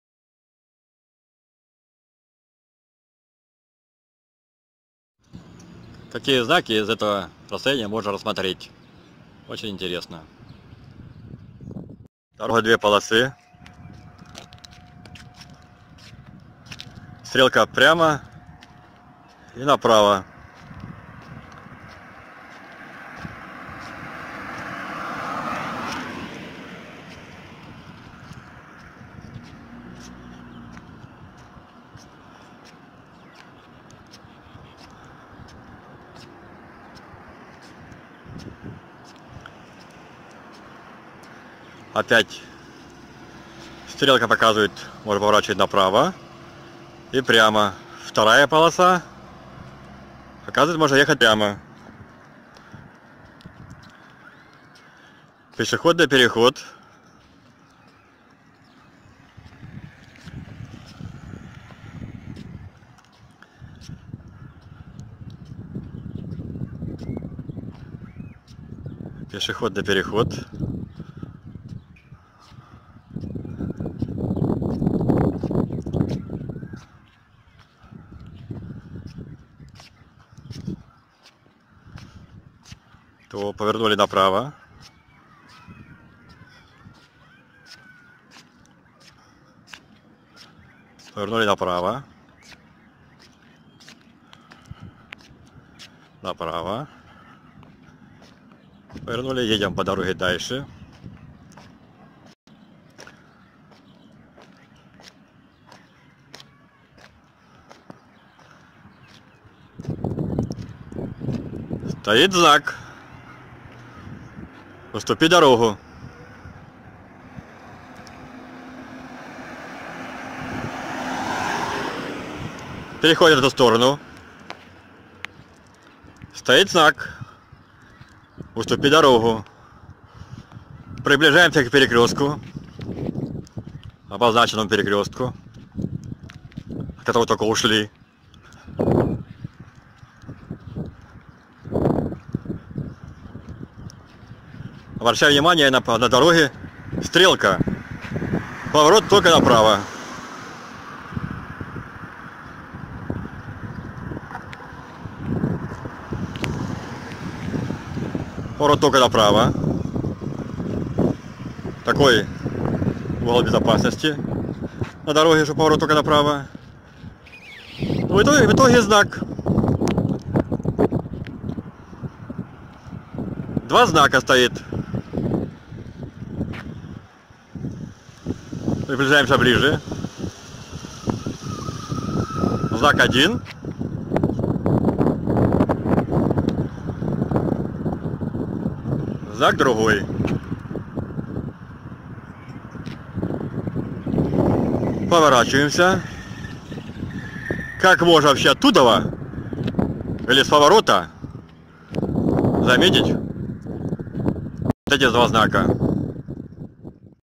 Арад (Израиль) Дорожная разметка .
Две полосы движения в одном направлении.Крайняя левая полоса разметка на дороге только прямо , крайне правая разметка на дороге прямо и направо.
Дорогая примыкающая разметка на дороге - стрелка - поворот только направо есть ещё знак за 100 метров " Уступи  дорогу" .Левый край дороги заретушован т.к только можно ехать по стрелке на право .
ИТОГ
По дороге при повороте на право стоит знак въезд запрещён , и при движении прямо стоит знак въезд запрещён.
ГДЕ ЛОГИКА - куда ехать???